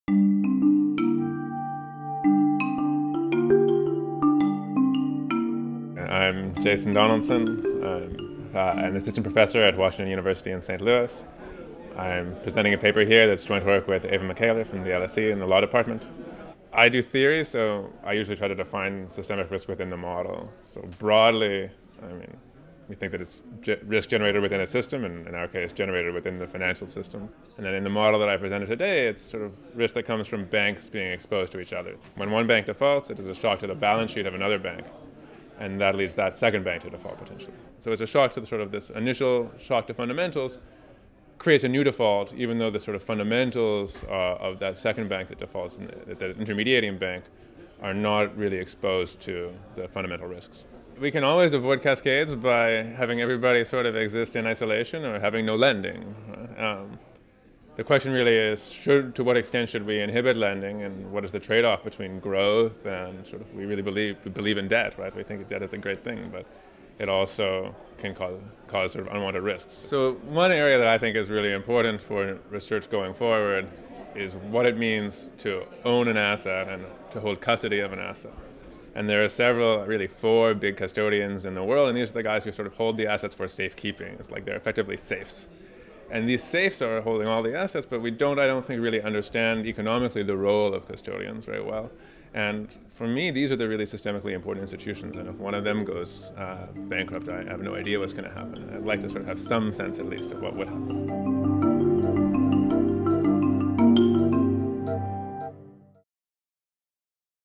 Perspectives on Systemic Risk conference: Interview with Jason Donaldson
Jason Donaldson, Assistant Professor, Olin Business School, Washington University in St Louis and Research Associate of Systemic Risk Centre, LSE, gives his views on systemic risk at the Perspectives on Systemic Risk conference on 16th October 2014